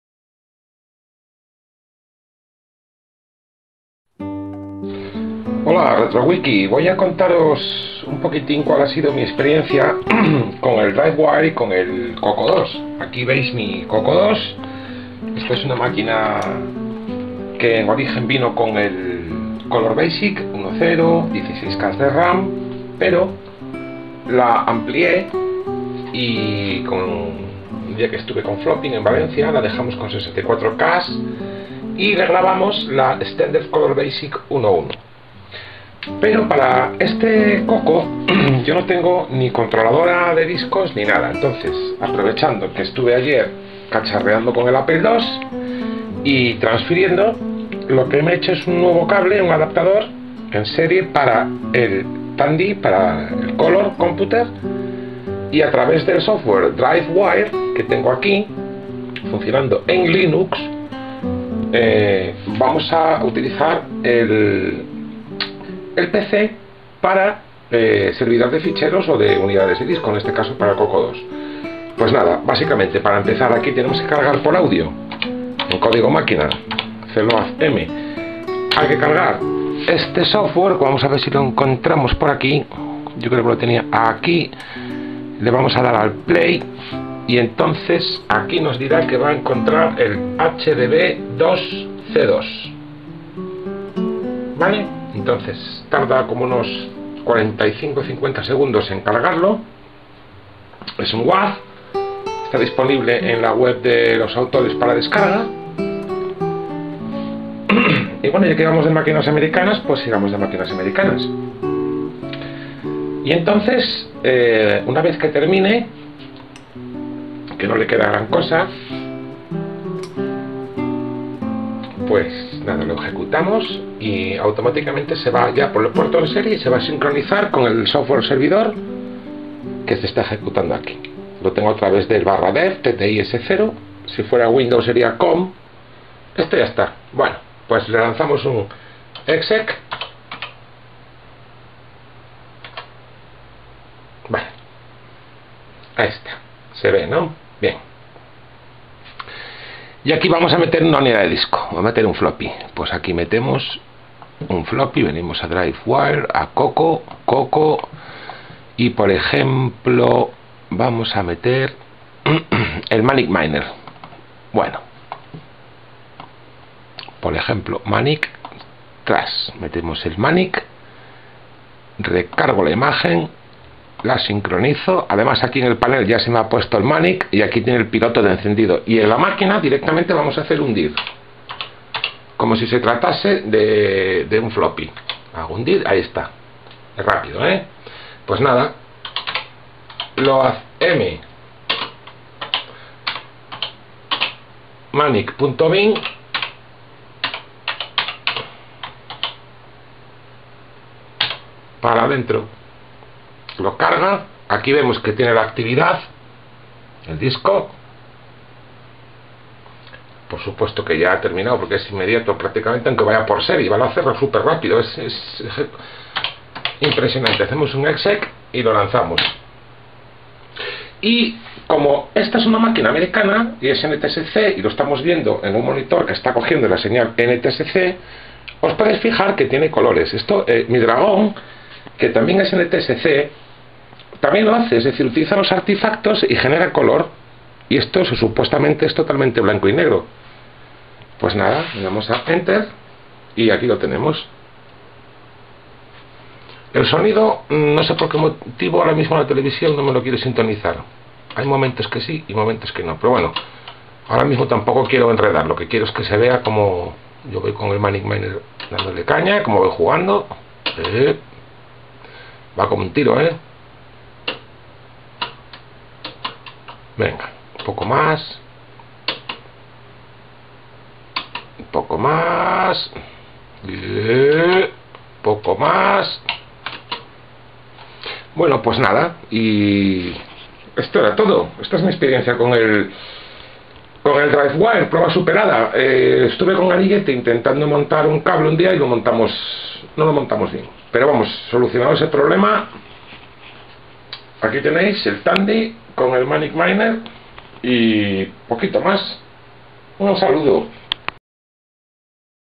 [RW] DriveWire Tandy CoCo2 con GNU/Linux
Conectando Tandy Color Computer 2 mediante DriveWire bajo GNU/Linux